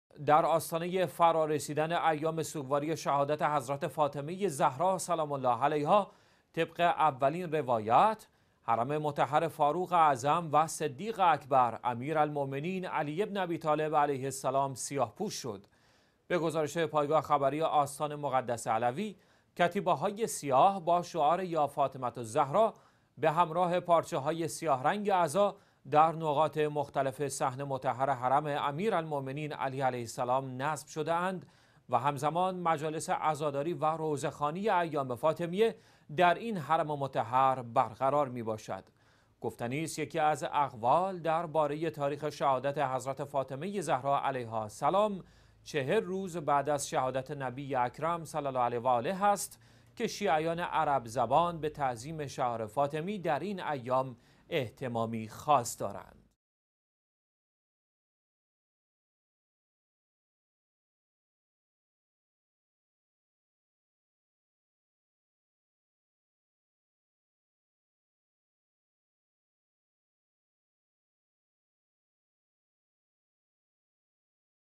سیاه پوشی حرم مطهر علوی، در آستانه ایام عزاداری فاطمیه اول/۱۹ دی ۱۳۹۵
سیاه پوشی حرم مطهر علوی، در آستانه ایام عزاداری فاطمیه اول

در آستانه فرا رسیدن ایام سوگواری شهادت حضرت_فاطمه_زهرا سلام الله علیها (طبق اولین روایت)؛ حرم مطهر فاروق_اعظم و صدیق اکبر امیرالمؤمنین علی بن ابیطالب علیه السلام سیاه پوش شد.

به گزارش پایگاه خبری آستان_مقدس_علوی، کتیبه های سیاه با شعار «یا فاطمه الزهرا» به همراه پارچه های سیاه رنگ عزا، در نقاط مختلف صحن مطهر حرم امیرالمومنین علی علیه السلام نصب شده اند و هم زمان مجالس عزاداری و روضه خوانی ایام_فاطمیه در این حرم مطهر برقرار می باشد.

گفتنی است یکی از اقوال در باره تاریخ شهادت حضرت فاطمه زهرا علیها سلام، 40 روز بعد از شهادت نبی_اکرم (صلی الله علیه و آله) است، که شیعیان عرب زبان به تعظیم شعائر_فاطمی در این ایام، اهتمامی خاص دارند.

الأحد - ٩ ربيع الثاني ١٤٣٨